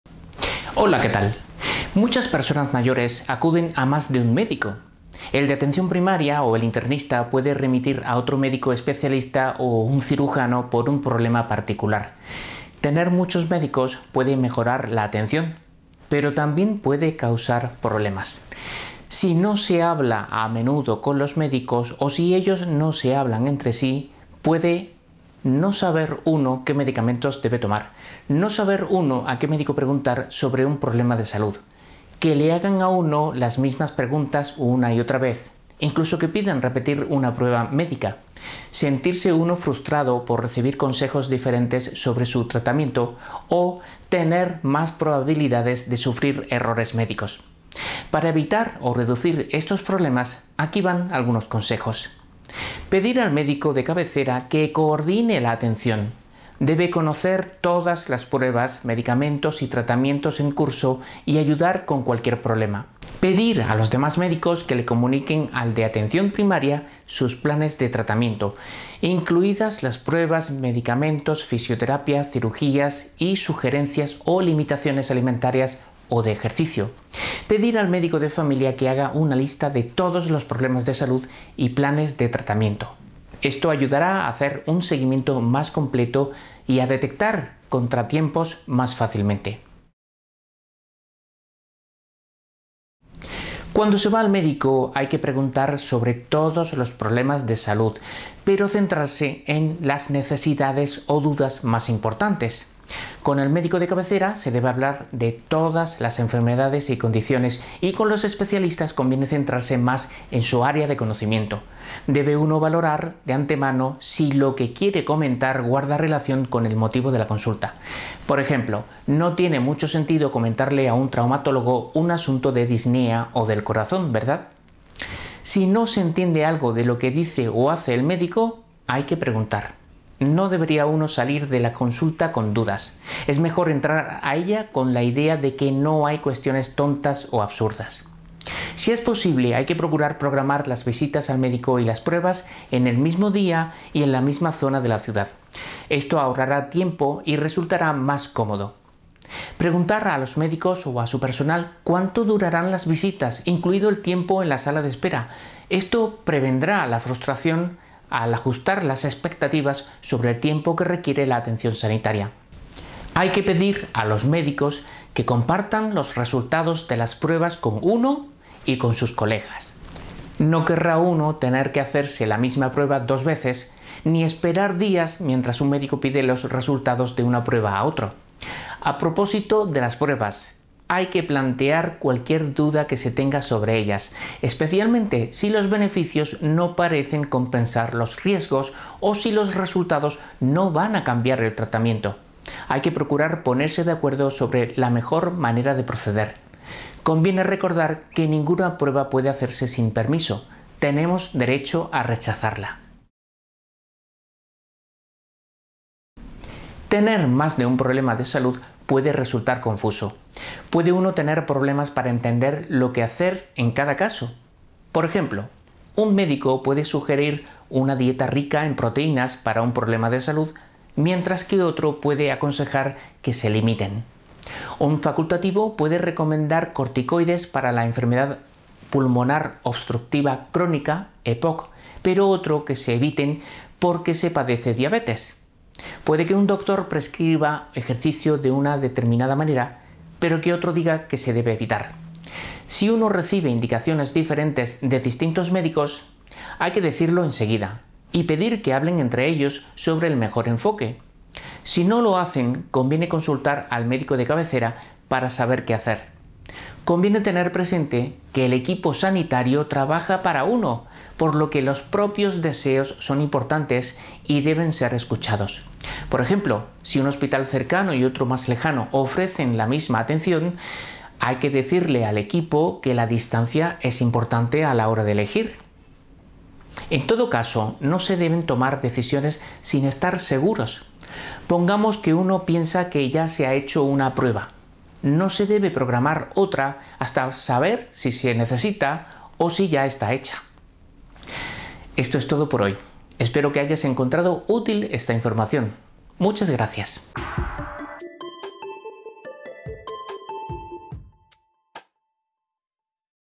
TENER MÁS DE UN MÉDICO ‐
Muchas personas mayores acuden a más de un médico. El de atención primaria o el internista, puede remitir a otro médico especialista o un cirujano por un problema particular.
Tener muchos médicos puede mejorar la atención, pero también causar problemas.
0:00 Introducción
1:31 Entenderse con el equipo sanitario
3:24 Obtener el asesoramiento adecuado